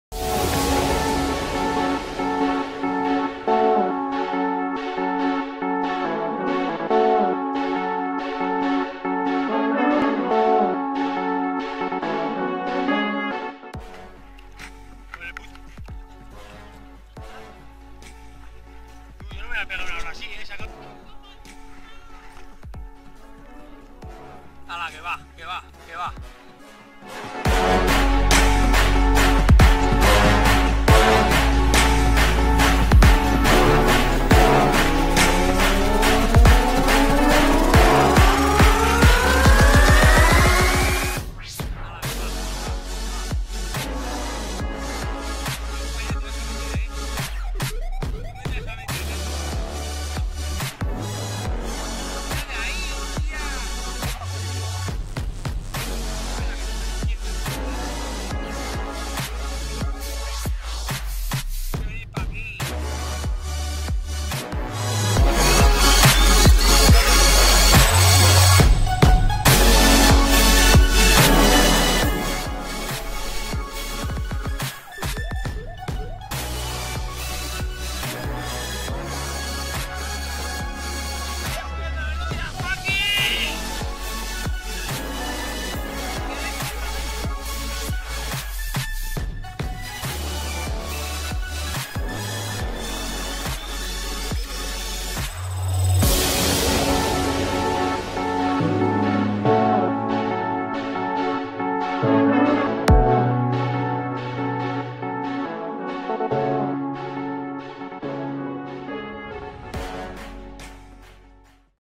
EBROHUNTERZ vs MONSTERDUCKS
Pelea sin descanso desde bellyboat con ayuda de hobie kayak.